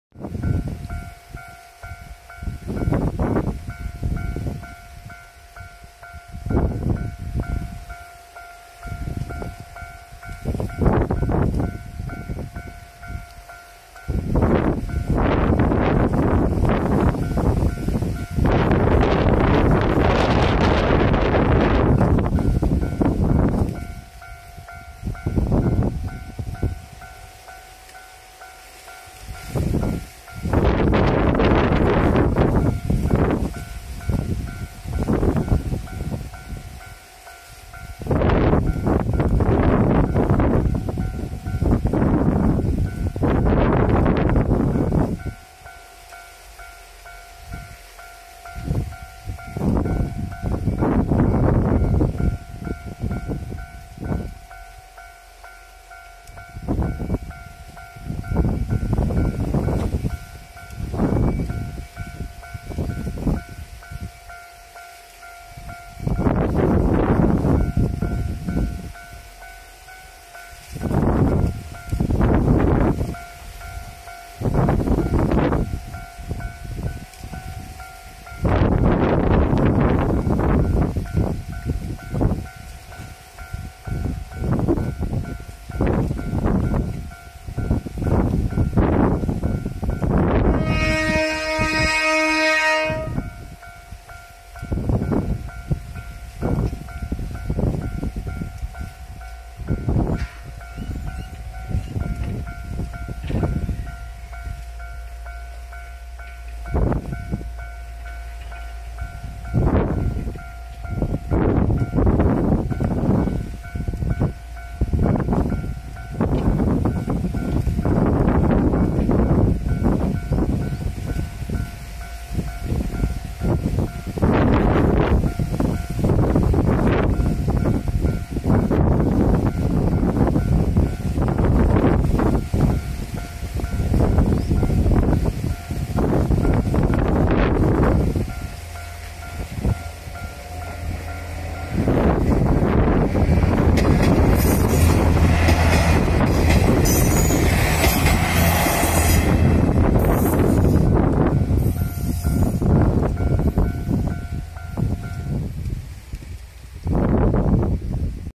西大山踏切🇯🇵日本最南端の踏切🚃鹿児島県
JR九州 指宿枕崎線
西大山〜薩摩川尻 駅間